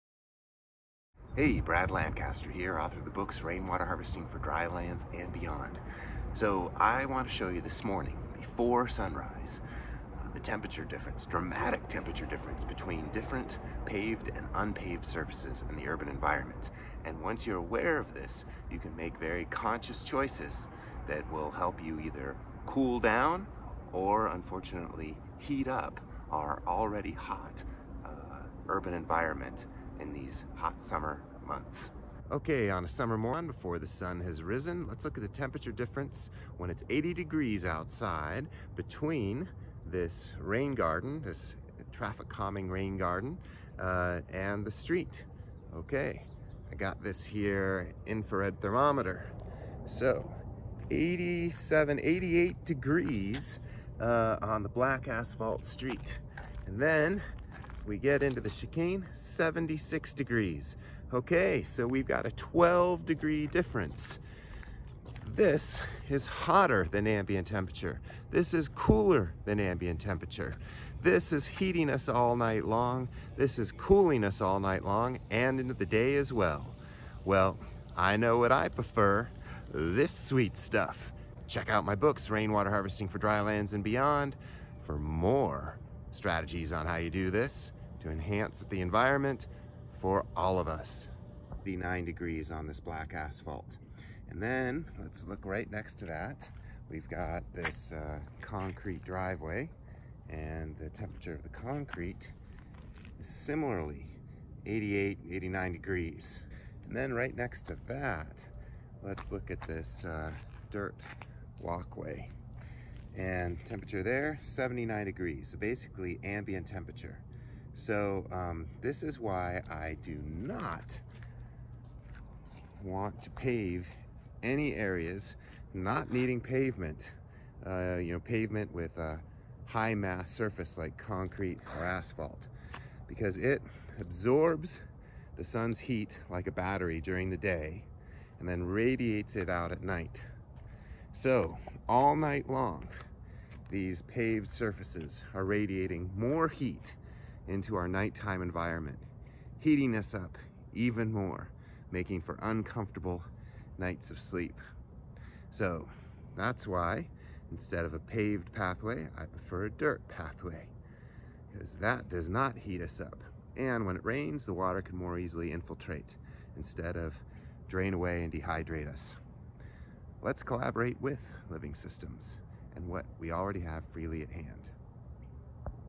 Comparing summer night time temperatures: Cooling rain gardens vs. heating pavement
Brad Lancaster shows us dramatic pre-sunrise temperature differences of paved and unpaved areas in the urban environment, so you can see what heats us up, and what cools us down (and by how much), on summer nights. So, you can make informed choices on how to transform your home, neighborhood, and street for more a more comfortable, joyous, and productive life for all.

I use a Fluke 62 MAX+ Handheld Infrared Laser Thermometer to take the temperature readings.

where you can buy the new full-color editions of Brad's award-winning books, "Rainwater Harvesting for Drylands and Beyond" at deep discount direct from Brad at: